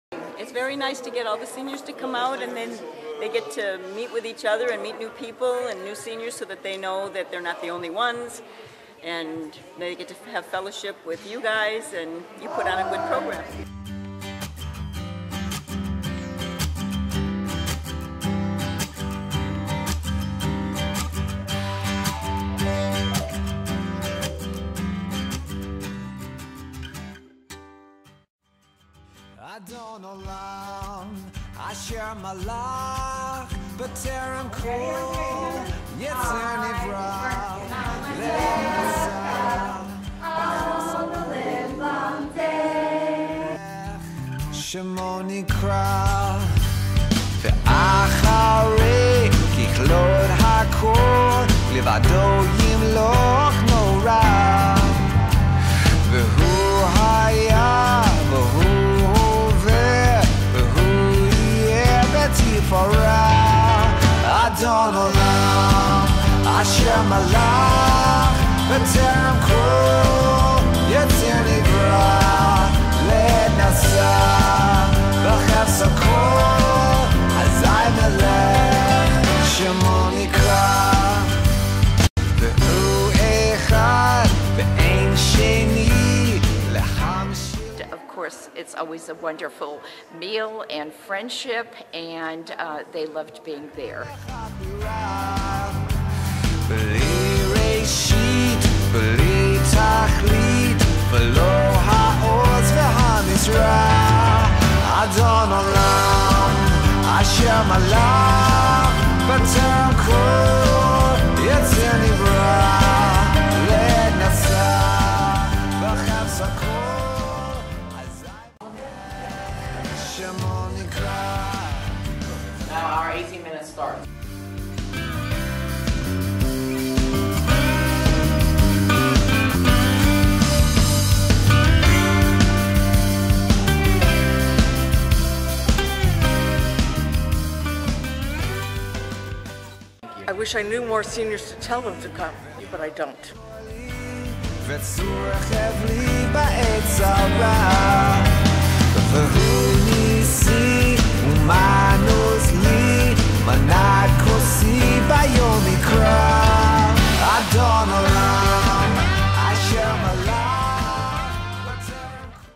SOS Year-end recap 2011-12
A brief recap highlighting our programs throughout the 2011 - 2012 year.
From High Holiday Candy Apple Dipping to our Shavuot Holiday program.

Thank you to our Volunteers and Supporters!